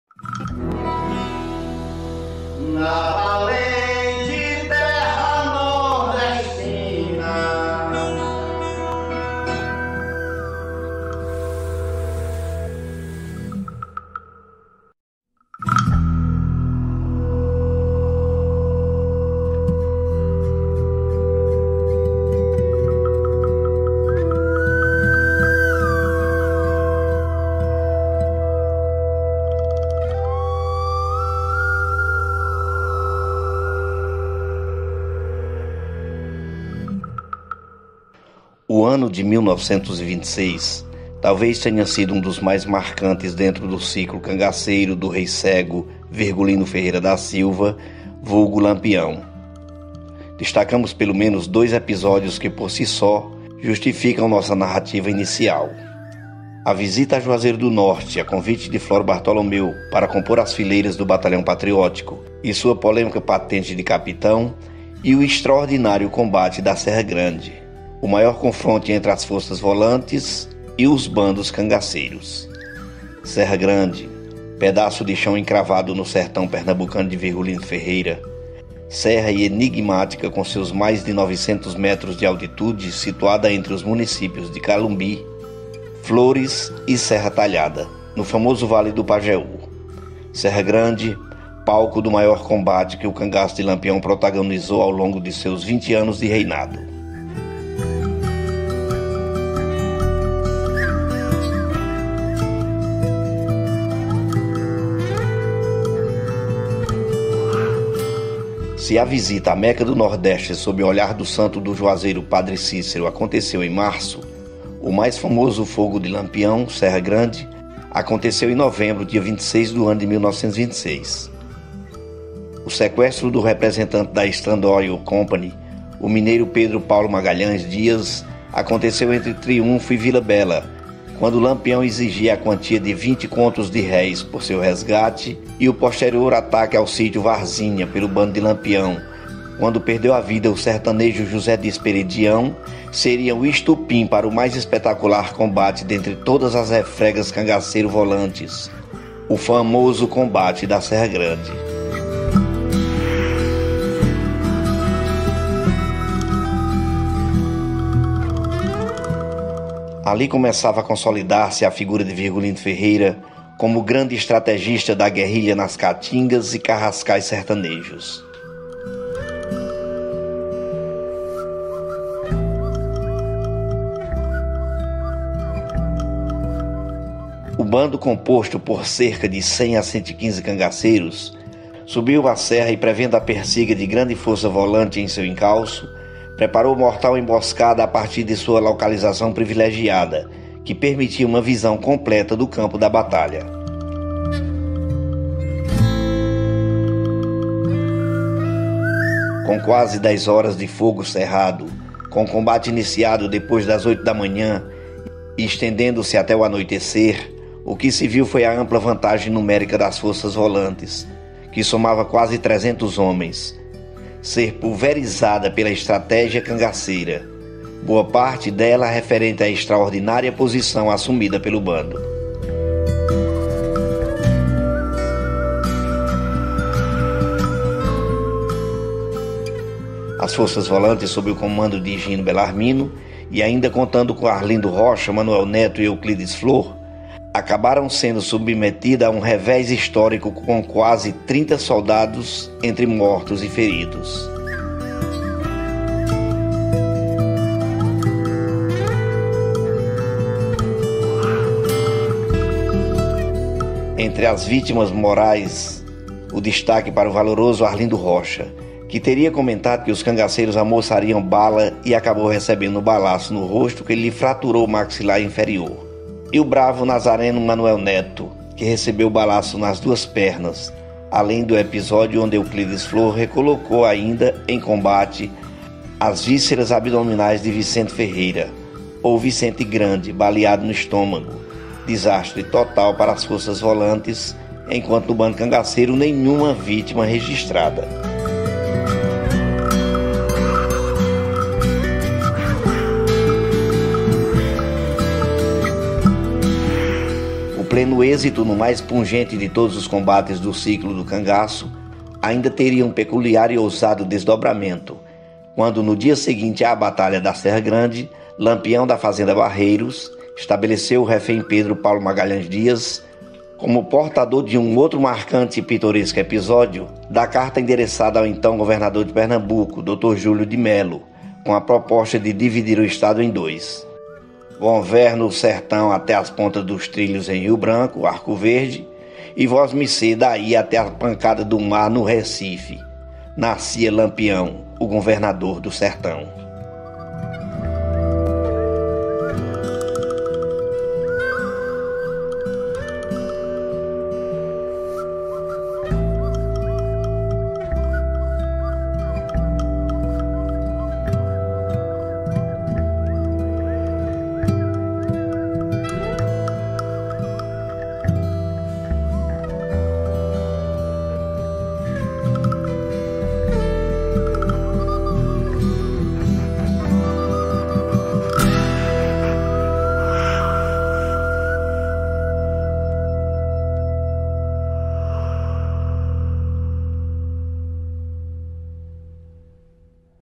LAMPIÃO E A BATALHA DA SERRA GRANDE
O ano de 1926 talvez tenha sido um dos mais marcantes dentro do ciclo cangaceiro do Rei Cego; Virgulino Ferreira da Silva, vulgo Lampião; destacamos pelo menos dois episódios que por si só, justificam nossa narrativa inicial: A visita a Juazeiro do Norte a convite de Floro Bartolomeu para compor as fileiras do Batalhão Patriótico e sua polêmica "patente" de capitão e o extraordinário combate da Serra Grande, o maior confronto entre as forças volantes e os bandos cangaceiros. Serra Grande, pedaço de chão encravado no sertão pernambucano de Virgulino Ferreira, serra enigmática com seus mais de 900 metros de altitude situada entre os municípios de Calumbi, Flores e Serra Talhada, no famoso Vale do Pajeú. Serra Grande, palco do maior combate que o cangaço de Lampião protagonizou ao longo de seus 20 anos de reinado